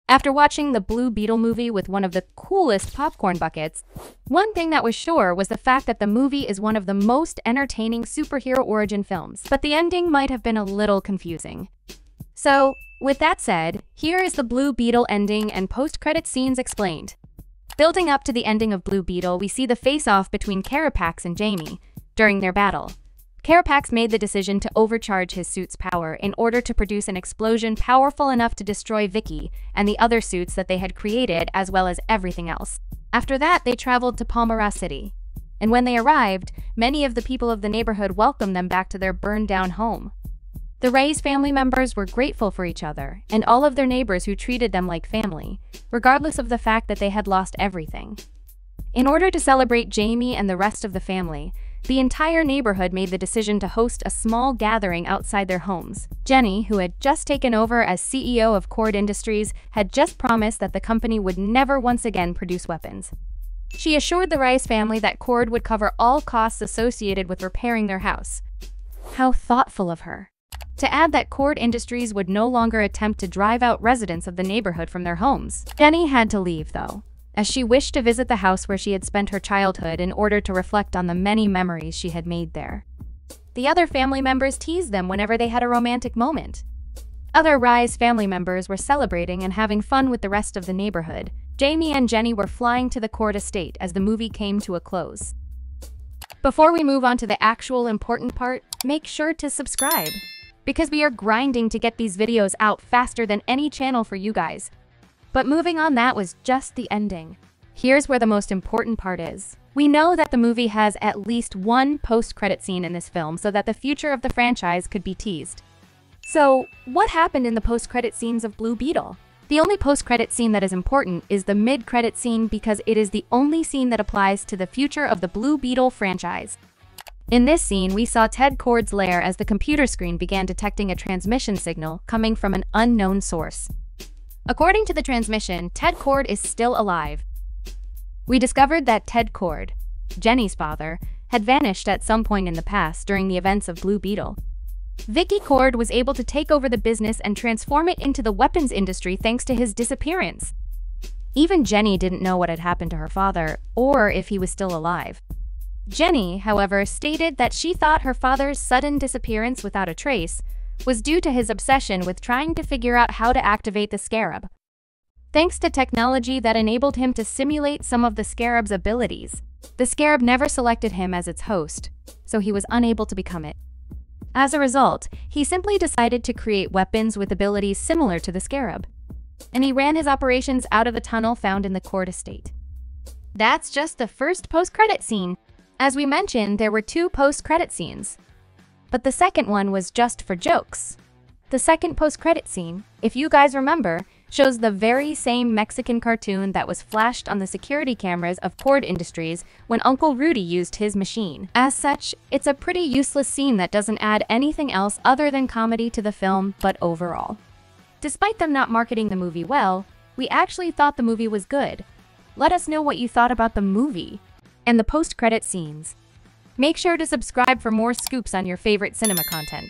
BLUE BEETLE ENDING EXPLAINED + POST CREDIT Scenes BREAKDOWN!
!! SPOLIERS AHEAD!! Dive into the Breakdown the ENDING of Blue Beetle and meaning of the post-credit scenes. We'll explore what Ted Kord's post credit scene appearance means for the movie's future. Plus, we'll dive into the details about the powerful scarab and, helping you fully grasp the exciting twists and turns that make up the Blue Beetle and Jaime universe.